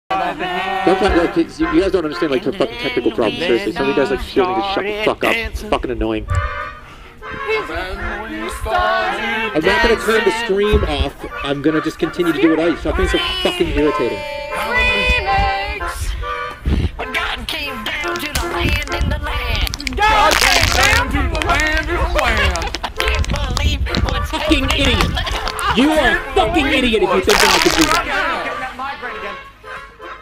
RAW Footage Of Jerma Reacting To Himself BREAK dancing
This Guy Is Way Too Spastic For Me.